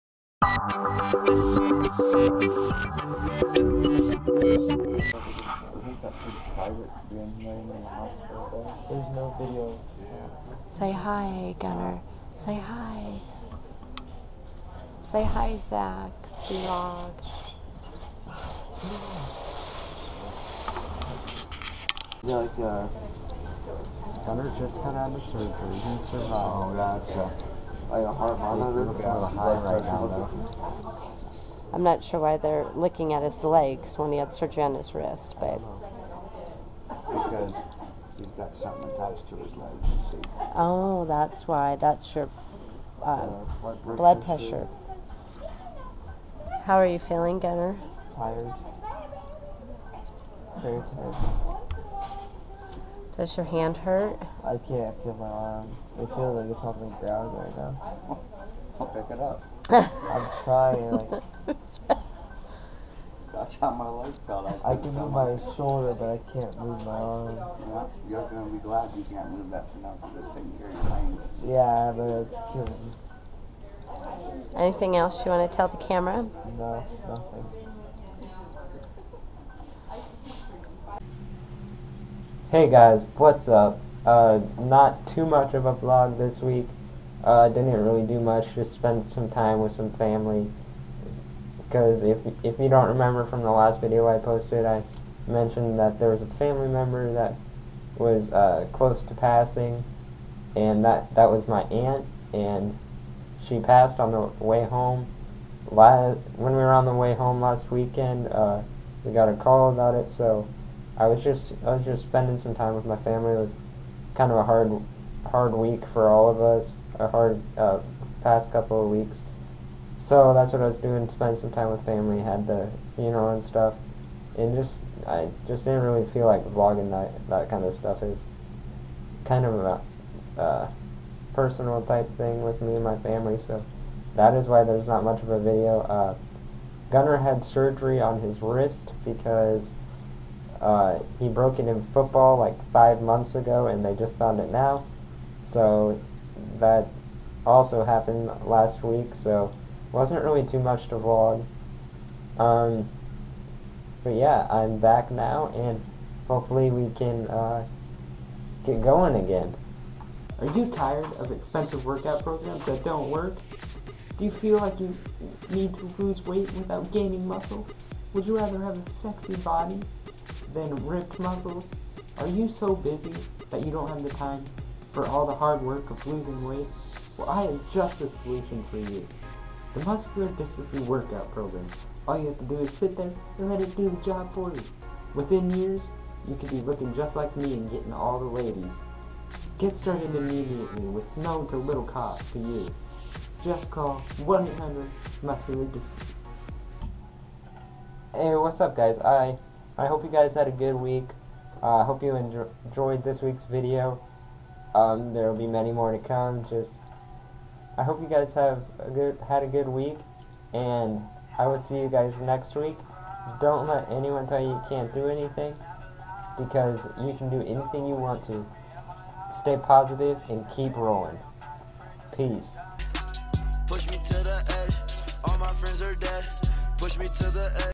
HE BROKE HIS WRIST!!!
What's good fam?! Not too much of a vlog this week, it was a rough week for me and my family! Gunnar had surgery and I found an amazing new workout program!!

sub_confirmation=1

Previous Vlog!